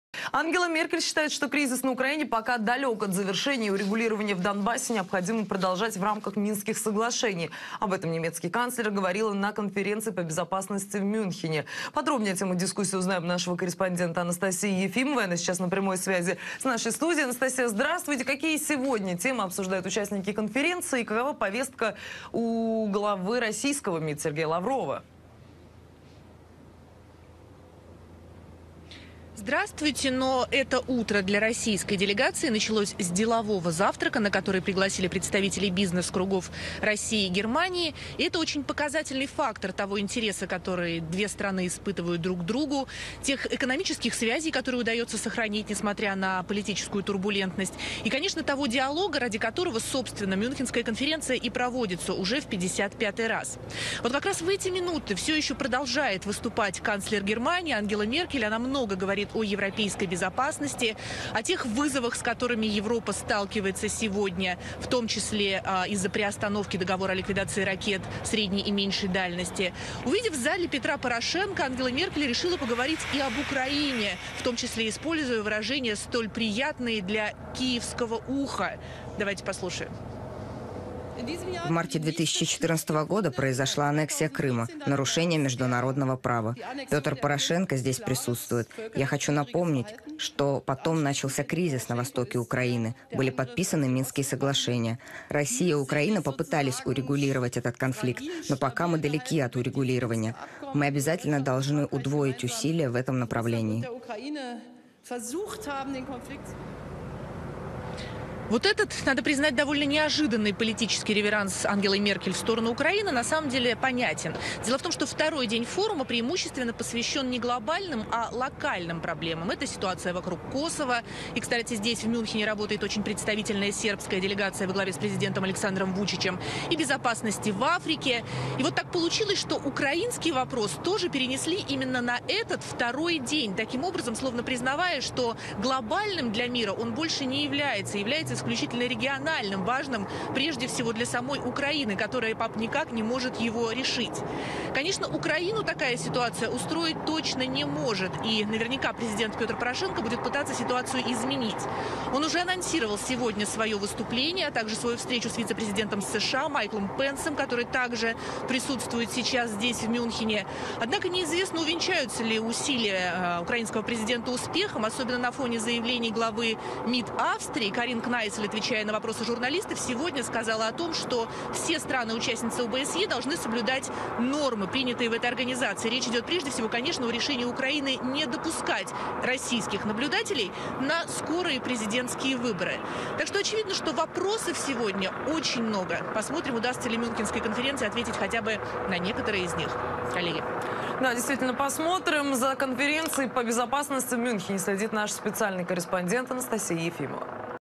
Меркель: нужно удвоить усилия по урегулированию кризиса в Донбассе
Ангела Меркель считает, что кризис на Украине пока далек от завершения и урегулирование в Донбассе необходимо продолжать в рамках Минских соглашений. Об этом немецкий канцлер говорила на конференции по безопасности в Мюнхене